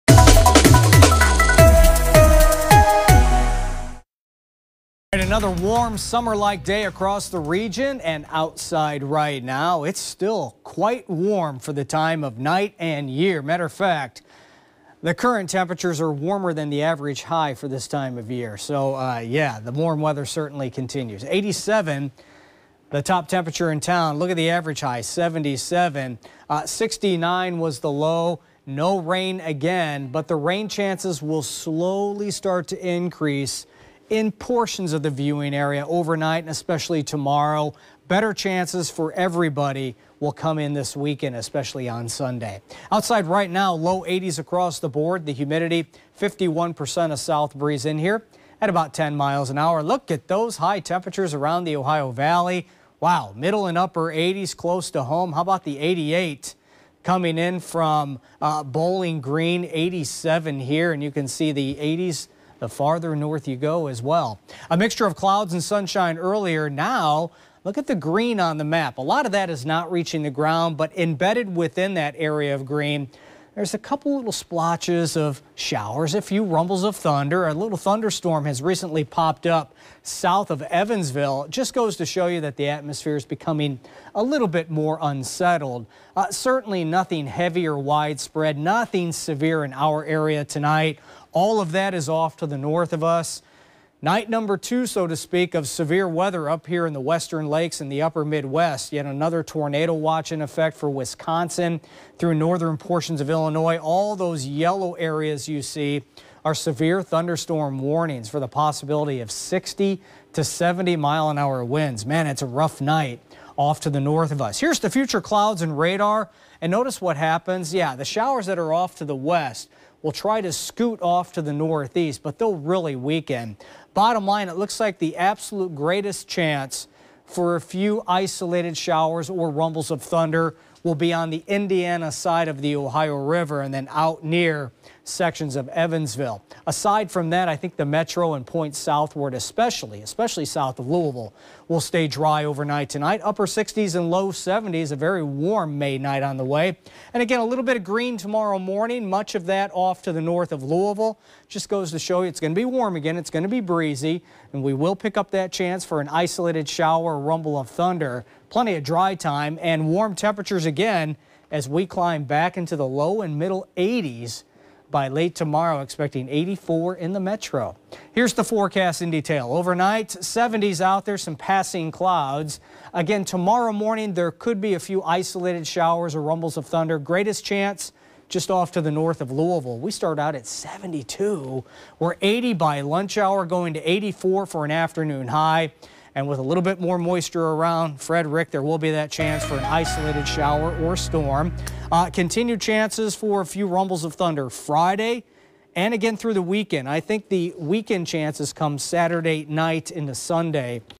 Isolated Showers possible overnight
Chief Meteorologist Jay Cardosi with your overnight forecast.